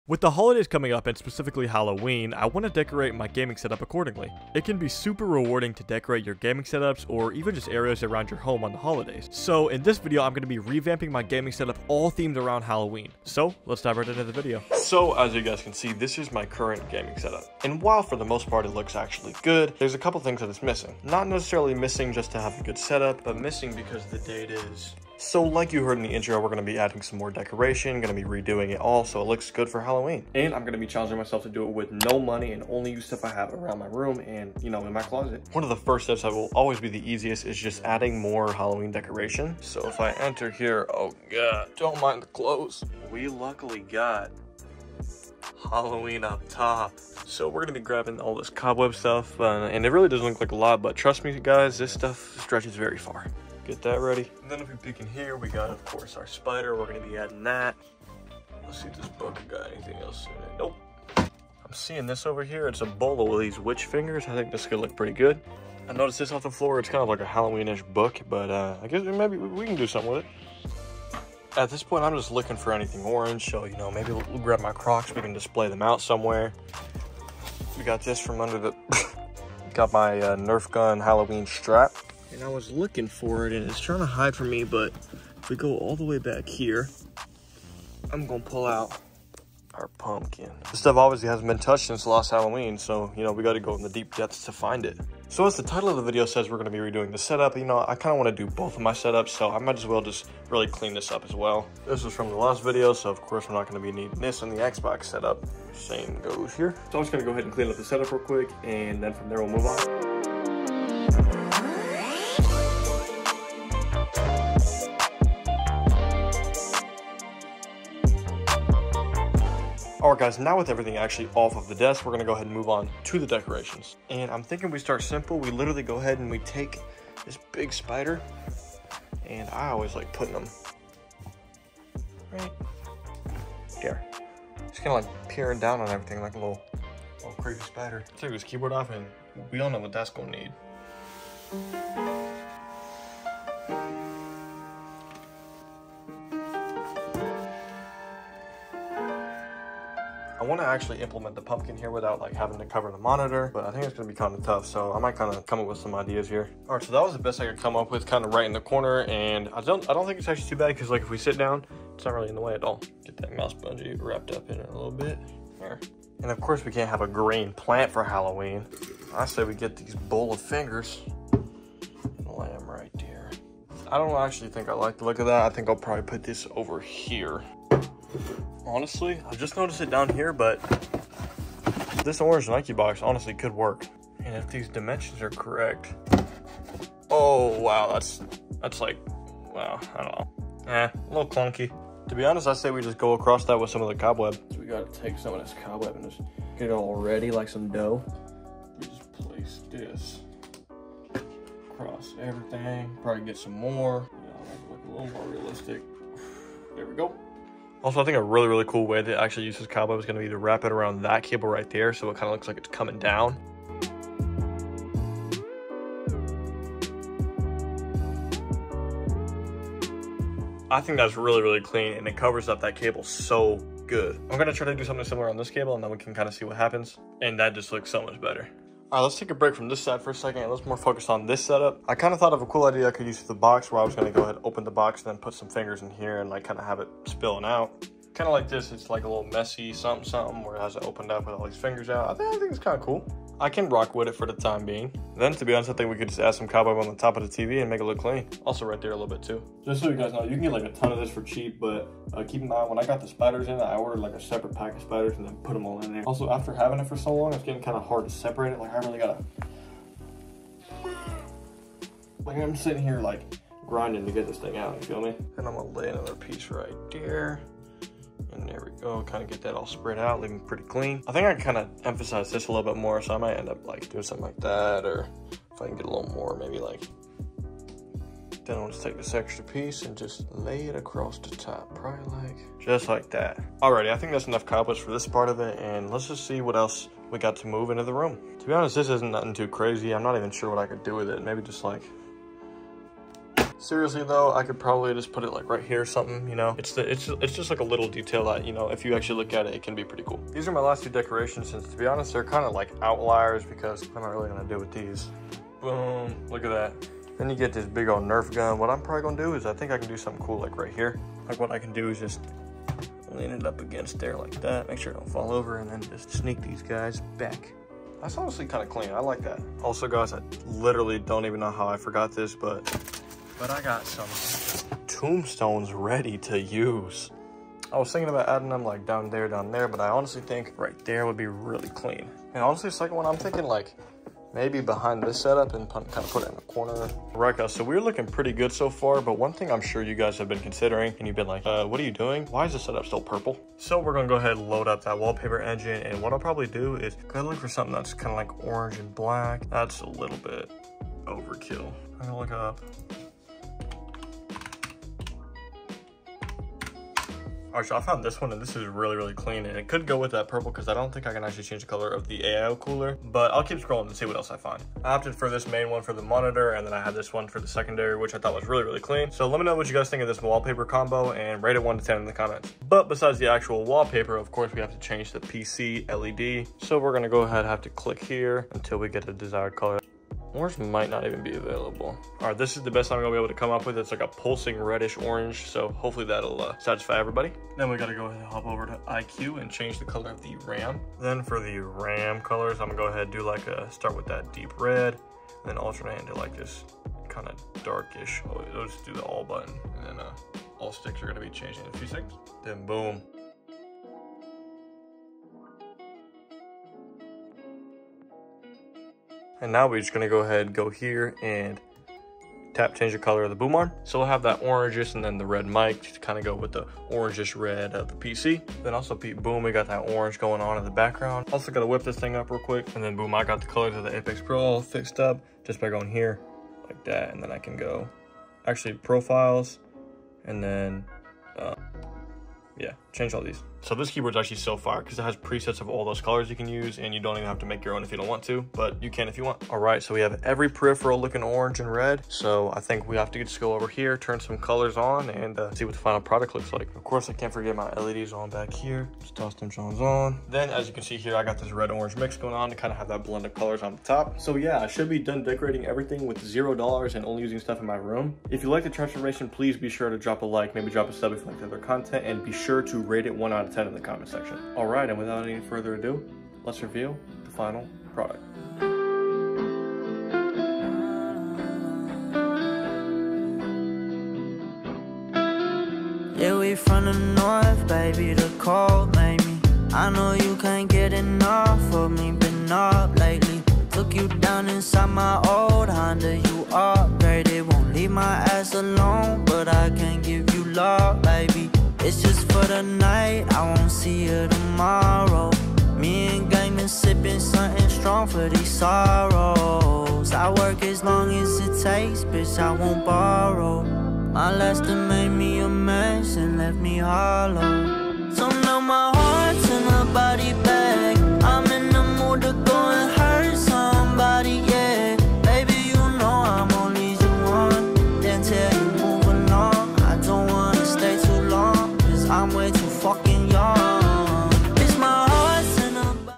I Revamped My Gaming Setup for Halloween!
In this video I take items from around my room and for no money spent, transform my gaming setup to be themed around Halloween!
SUPPORT MY THUMBNAIL EDITOR**
ALL MY PC SPECS ARE IN THE PC PARTS PLAYLIST ON MY CHANNEL IF YOU ARE CURIOUS